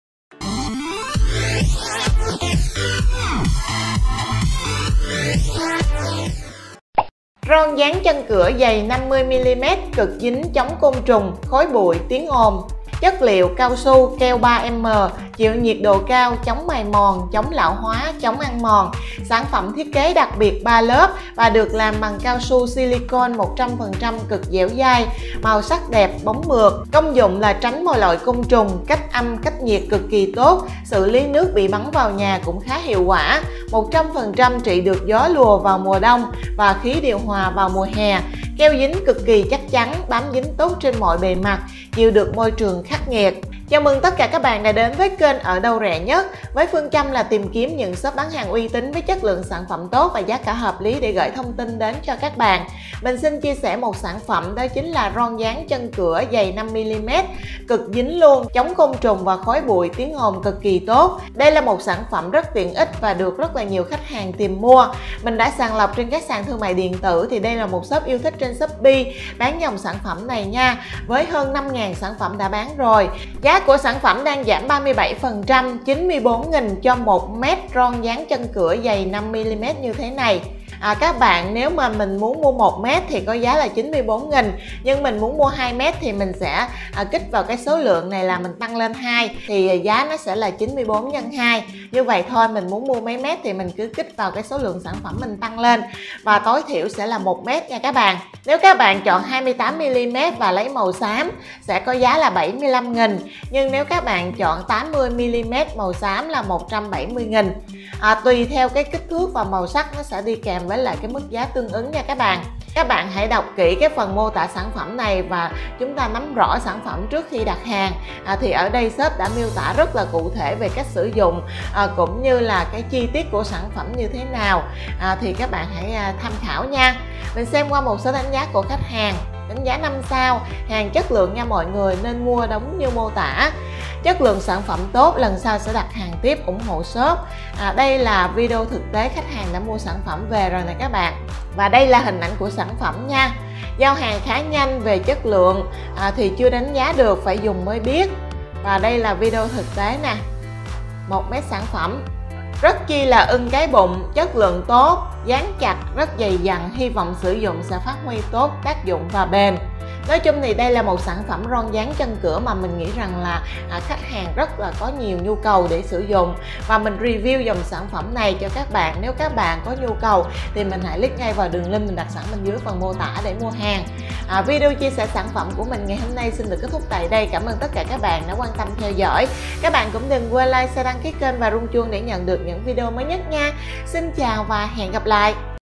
Ron Dán Chân Cửa Cực Dính Chống Côn Trùng, Khói Bụi, Tiếng Ồn giá rẻ nhất Shopee, Lazada, Tiki
Mua Ron Dán Chân Cửa Dày 5MM Cực Dính Chống Côn Trùng, Khói Bụi, Tiếng Ồn giá rẻ nhất trên shopee:

Ron Dán Chân Cửa Dày 5MM Cực Dính Chống Côn Trùng, Khói Bụi, Tiếng Ồn - Doorseal (Vietnam)
Note: SỐ LƯỢNG 1 LÀ 1 MÉT
 Ví dụ: bạn mua số lượng 2 thì shop sẽ cắt cho bạn cuộn dài  2 mét

Thông số kỹ thuật:
- Độ dày: 5mm
- Chất liệu: cao su, keo 3M
- Chịu nhiệt độ cao, chống mài mòn, chống lão hóa, chống ăn mòn
- Sản phẩm thiết kế đặc biệt 3 lớp và  được làm bằng cao su silicon 100% cực dẻo dai, màu sắc đẹp, bóng mượt

Công dụng:
- Tránh mọi loại côn trùng

- Cách âm, cách nhiệt cực kỳ tốt.

- Xử lý nước bị bắn vào nhà cũng khá hiệu quả.

- 100% trị được gió lùa vào mùa đông và thoát khí điều hòa vào mùa hè.

- Keo dính cực kỳ chắc chắn, bám dính tốt trên mọi bề mặt, chịu được trong môi trường khắc nghiệt.

Bước 1: Vệ sinh sạch bề mặt cần dán.
Bước 2: Đo chiều dài chân cửa.
Bước 3: Cắt ron khít với chiều dài cửa đã đo. Quý khách có thể cắt dài ra 1 chút.
Bước 4: Dán ron cẩn thận lên bề mặt, miết chắc chắn, tránh gỡ ra dán lại nhiều lần.

- Ron 40mm: che được khe hở nhỏ hơn 20mm
- Ron 50mm: che được khe hở nhỏ hơn 30mm